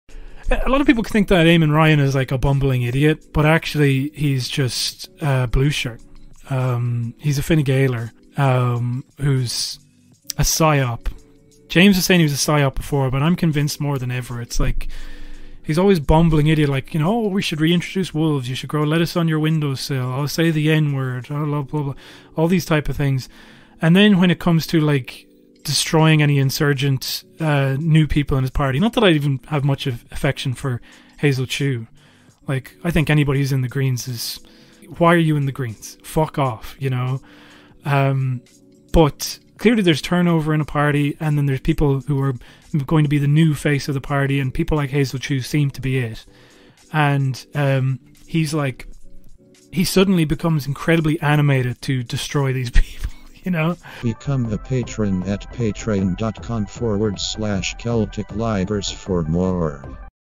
#138 don't be mean to me
The Liger talks:

-Mark Paul going after the family, -Anthony Leddin being a shithead,-Pascal Donohue's ingenious idea to catalogue vacant properties!

All yours and more for a 5er a month